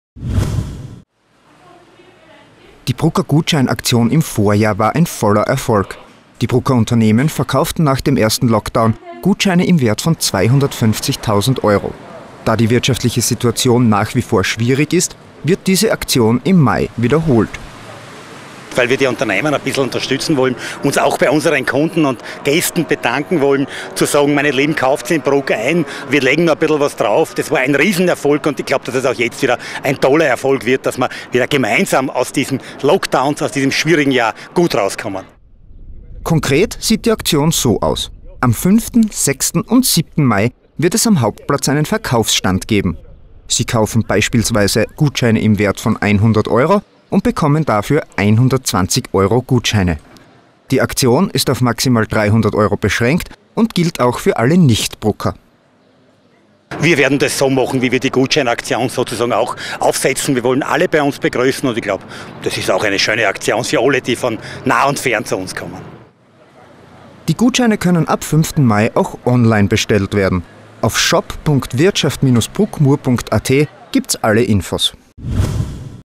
Brucker Gutscheinaktion wird wiederholt
Die Brucker Gutscheinaktion im Vorjahr war ein voller Erfolg. Die Unternehmen verkauften nach dem ersten Lockdown Gutscheine im Wert von 250.000 Euro. Im Mai wird diese Aktion wiederholt.